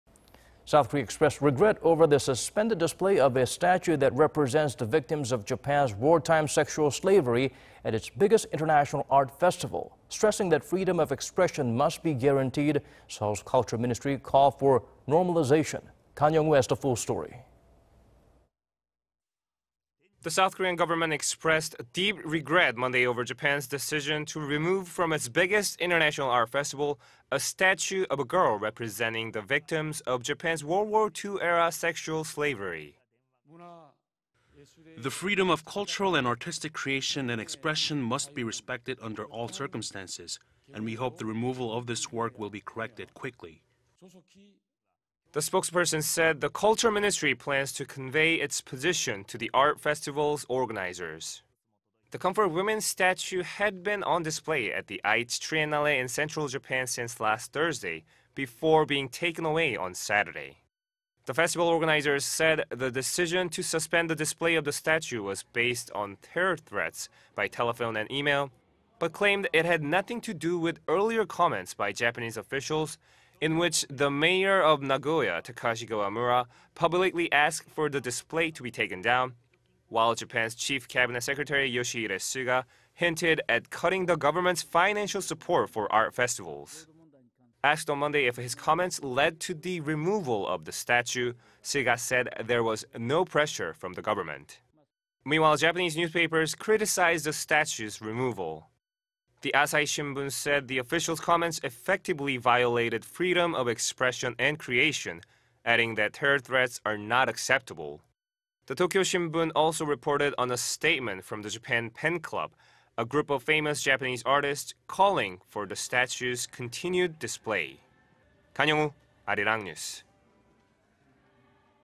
S. Korea expresses regret over Japan's removal of 'comfort women' statue at int'l arts festival
문체부 "일본 소녀상 전시 중단 매우 유감... 창작과 표현의 자유 보존돼야"

South Korea expressed regret over the suspended display of a statue that represents the victims of Japan's wartime sexual slavery at its biggest international art festival.
Stressing that freedom of expression must be guaranteed,... Seoul's culture ministry called for normalization.
Kan Hyeong-woo has the full story.
The South Korean government expressed deep regret Monday over Japan's decision to remove from its biggest international art festival... a statue of a girl representing the victims of Japan's World War II-era sexual slavery.
"The freedom of cultural and artistic creation and expression must be respected under all circumstances,... and we hope the removal of this work will be corrected quickly."
The spokesperson said the culture ministry plans to convey its position to the art festival's organizers.
The comfort women statue had been on display at the Aichi Triennale in central Japan since last Thursday... before being taken away on Saturday.
The festival organizers said the decision to suspend the display of the statue was based on "terror threats" by telephone and e-mail, but claimed it had nothing to do with earlier comments by Japanese officials... in which the mayor of Nagoya, Takashi Kawamura, publicly asked for the display to be taken down... while Japan's Chief Cabinet Secreatry, Yoshihide Suga, hinted at cutting the government's financial support for art festivals.
Asked on Monday if his comments led to the removal of the statue,... Suga said there was no pressure from the government.

Meanwhile... Japanese newspapers criticized the statue's removal.
The Asahi Shimbun said the officials' comments effectively violated freedom of expression and creation,... adding that "terror threats" are not acceptable.
The Tokyo Shimbun also reported on a statement from the Japan P.E.N. Club - a group of famous Japanese artists - calling for the statue's continued display.
Aichi Governor Hideaki Omura, held a press conference regarding the controversial decision.
Omura harshly criticized Nagoya Mayor Kawamura over his objection to the exhibit, saying such a move is very likely in violation of the Constitution.